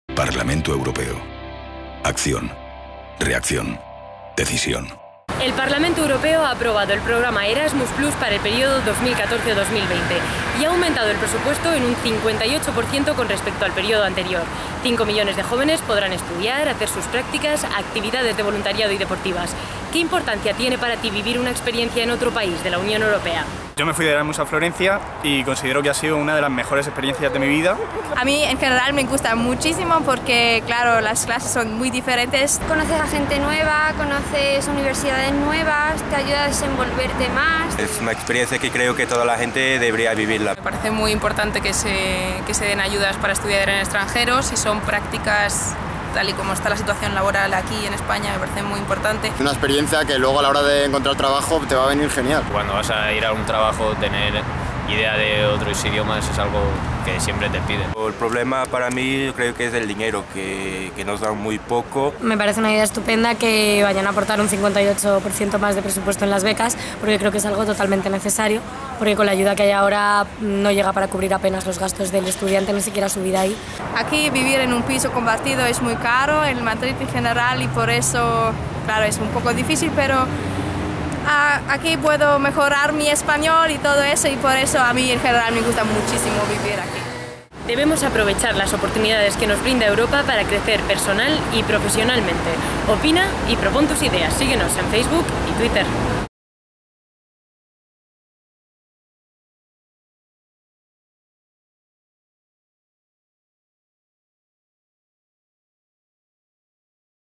TÚ OPINAS: ¿Qué importancia tiene para tí ERASMUS?
El Parlamento Europeo ha aprobado ERASMUS+ para el periodo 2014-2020 con un 58% más de presupuesto destinado al programa de movilidad Erasmus con respecto al periodo anterior (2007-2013).